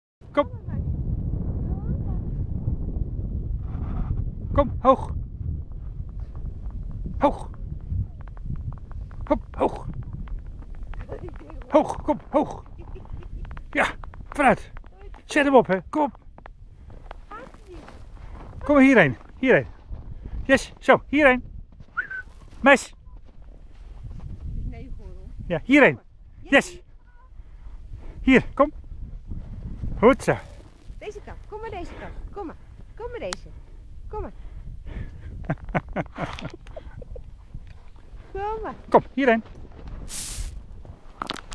Valentines day "Snow heart"
Our dog ( bouvier des flandres ) made a hudge snowheart in the fresh snow of the Italien Dolomites ( 2500 mts above sealevel)